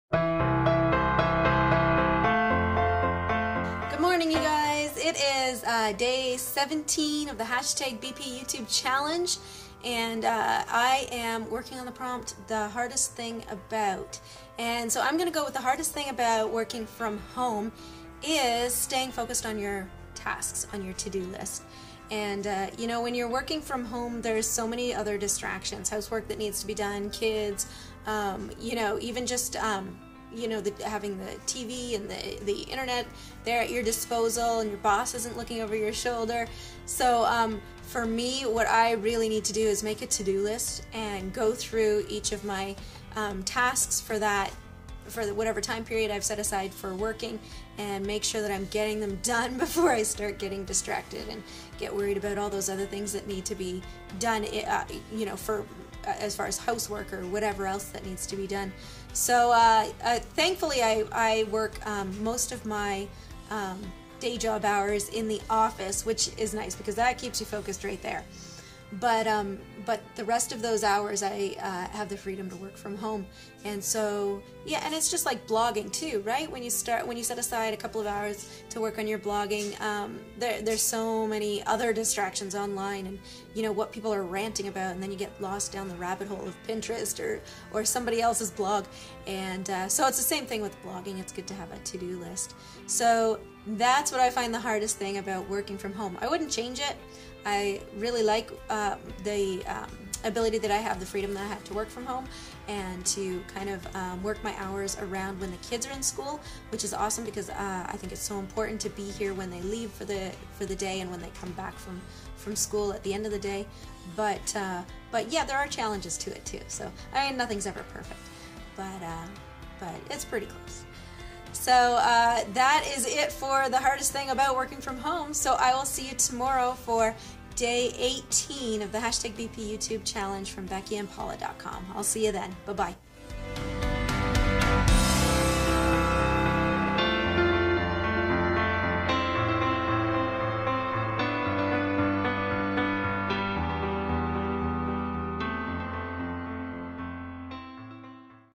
BPYoutubeChallenge - Day 17 - The Hardest Part About ... Working From Home
Today Megan answers the question "What Is The Hardest Part About Working From Home?" as part of the bpyoutubechallenge with BeckyAndPaula.com.